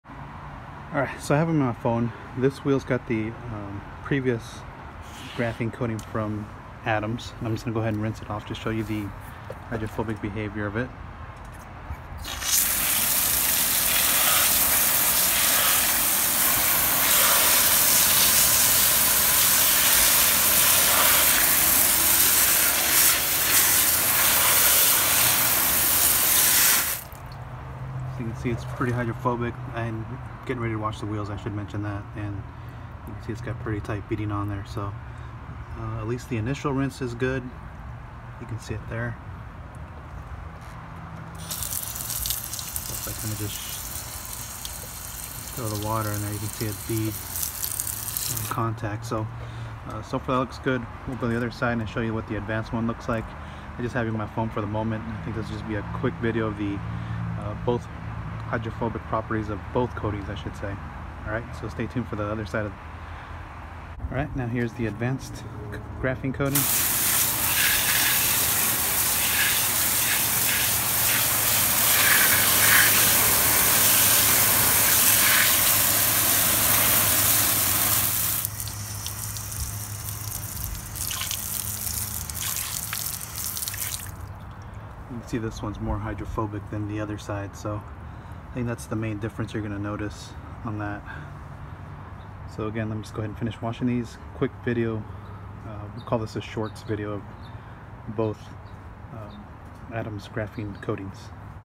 Adams Graphene vs Adams Advanced Graphene Hydrophobic Behavior
Comparison of the hydrophobic behavior  between the Adams Graphene Ceramic Coating and the Adams Advanced Graphene Ceramic Coating.

Adams Graphene Ceramic: (0:00)
Adams Advanced Graphene Ceramic: (1:04)